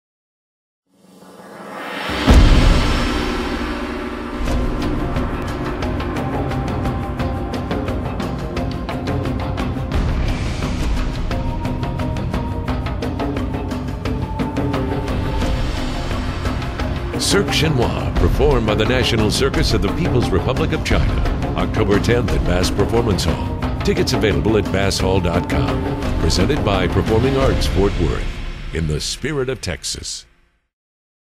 CIRQUE CHINOIS coming to Bass Performance Hall Oct. 10!
Direct from Beijing, the National Circus of the People's Republic of China was founded in 1953, making it one of the longest-running and most distinguished circus troupes in China.

The ensemble is currently on tour, taking its "Cirque Chinois" show to audiences across the globe.

The National Circus of the People's Republic of China is one of the most acclaimed acts in China. The show is a combination of trapeze artistry, balancing acts, juggling and dance, all set to beautiful music.